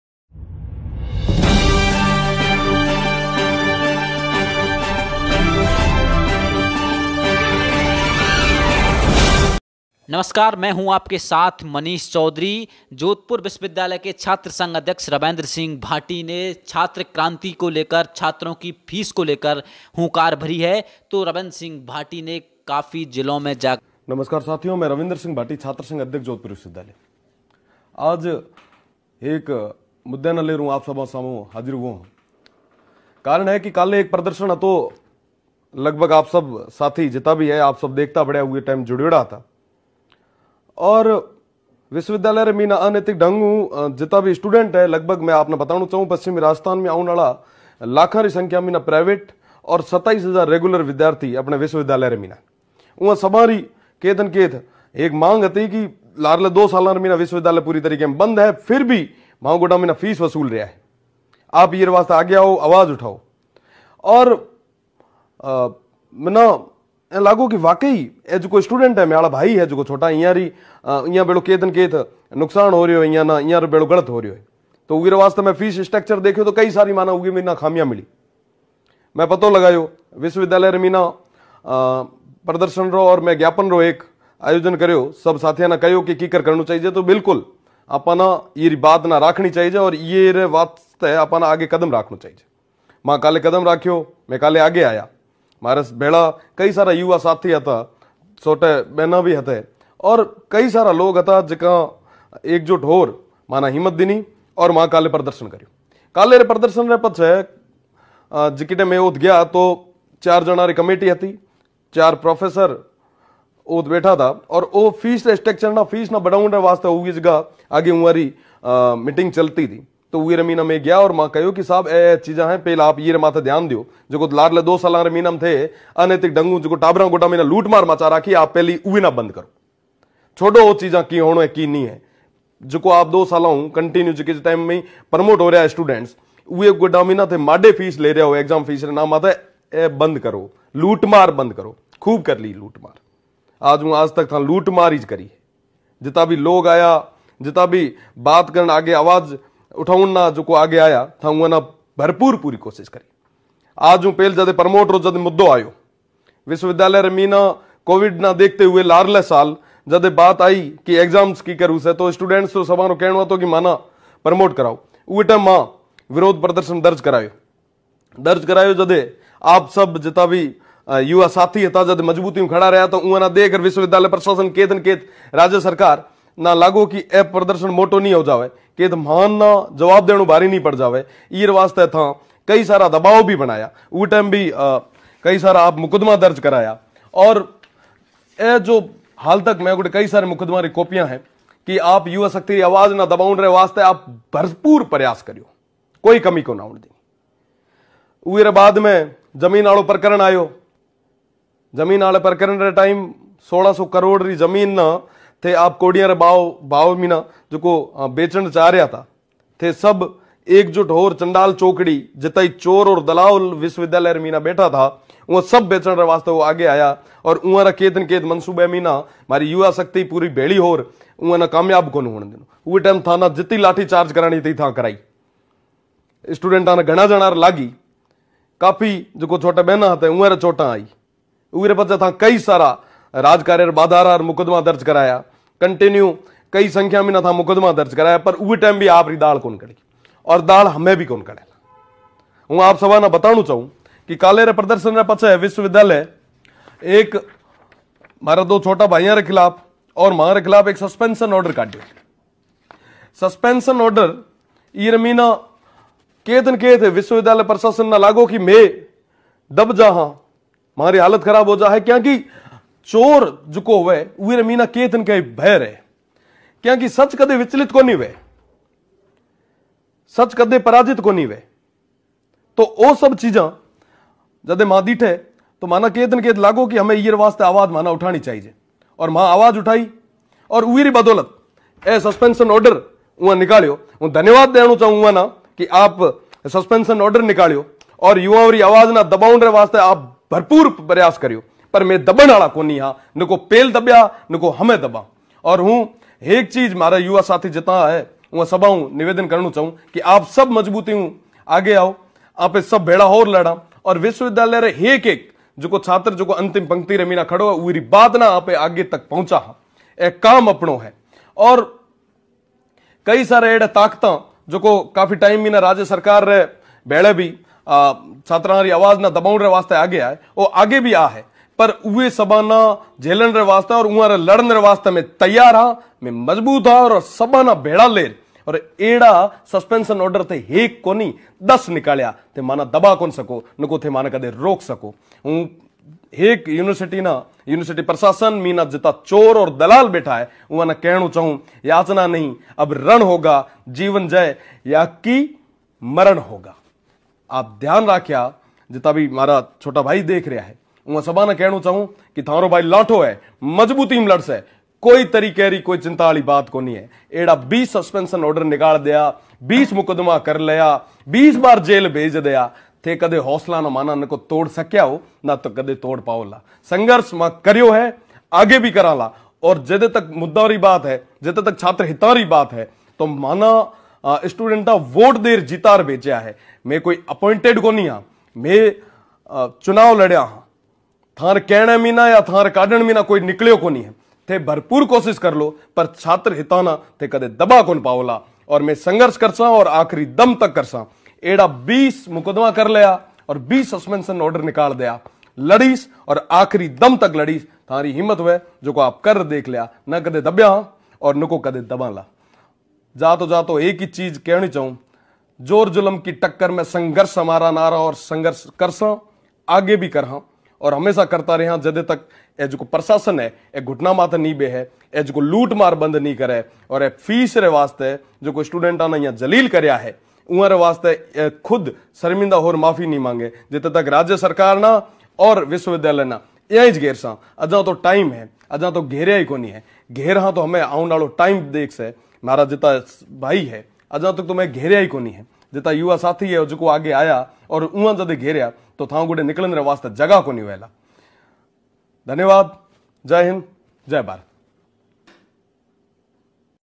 छात्रसंघ अध्यक्ष रविंद्र सिंह भाटी की गहलोत सरकार को खुली चेतावनी
13 सितंबर को विधानसभा घेराव करेंगे भाटी देखिए मनीष चौधरी  की रिपोर्ट